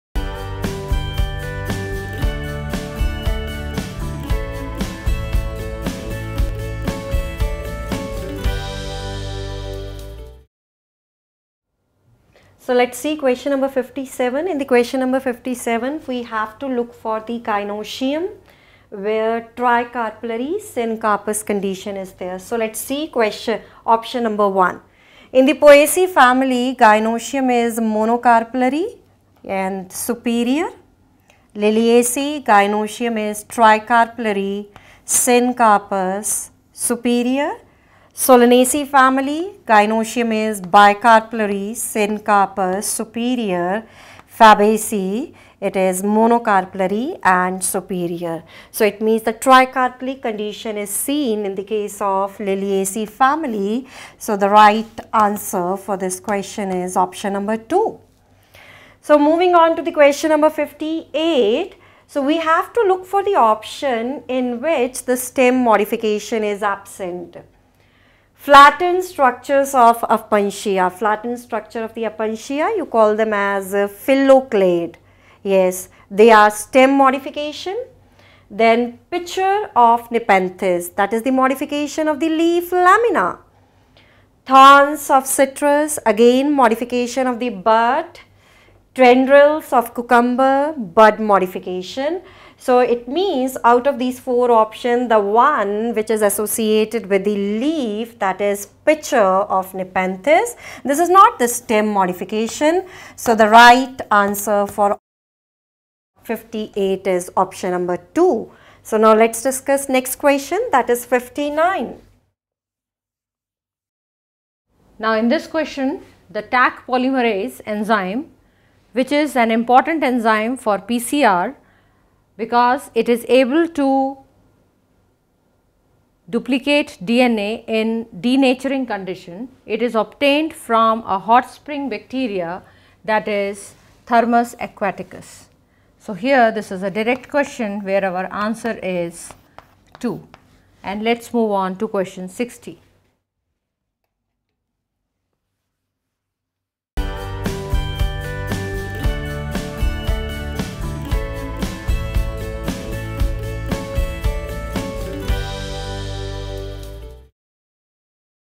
AIPMT 2016 Biology Solution Q. 57-59 Aakash (NEET Phase-I)
Find AIPMT 2016 Biology Video Solution (NEET Phase-I) from Question No. 57-to-59(Code-Y) by Aakash Institute.